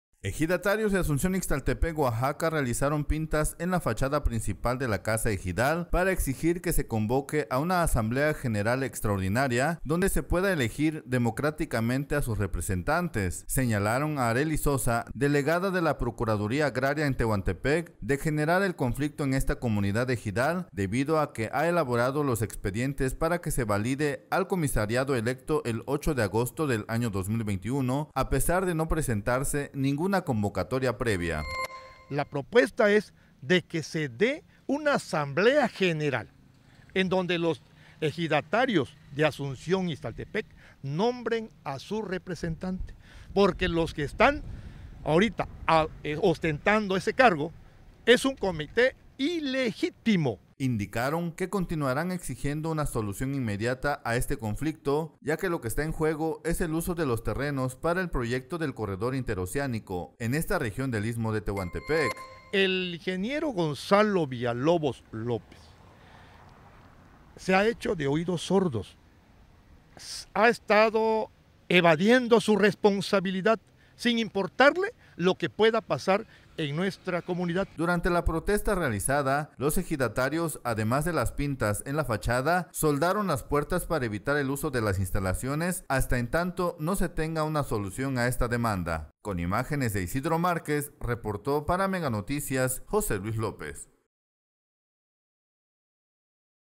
FT PROTESTA EJIDATARIOS IXTALTEPEC
Ejidatarios de Ixtaltepec, Oaxaca, realizaron pintas en la Casa Ejidal, exigen nueva elección para nombrar al Comisariado Ejidal.